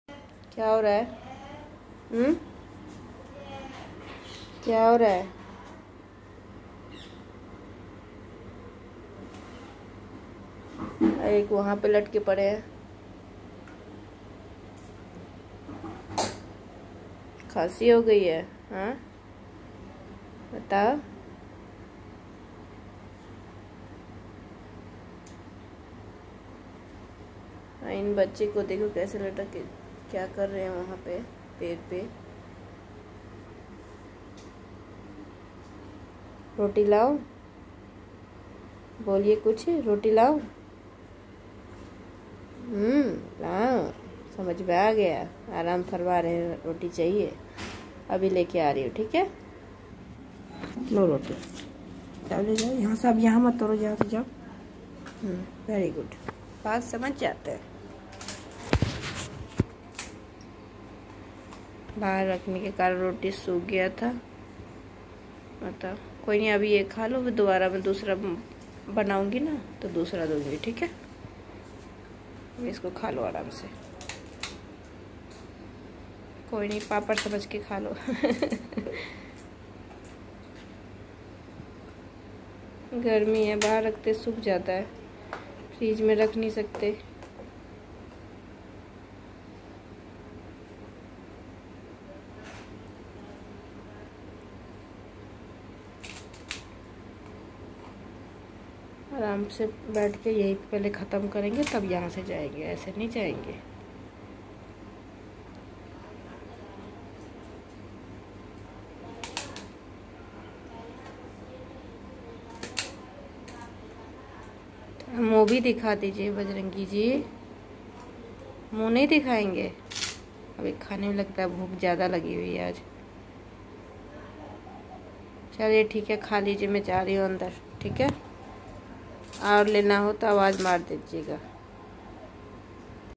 Roti Sukh Gayi Hai Tabhi Bajrangi usko Papad samajhkar kha rahe hain🤗🙏😁❤️bajrangi
Roti Sukh Gayi Hai Tabhi Bajrangi usko Papad samajhkar kha rahe hain🤗🙏😁❤️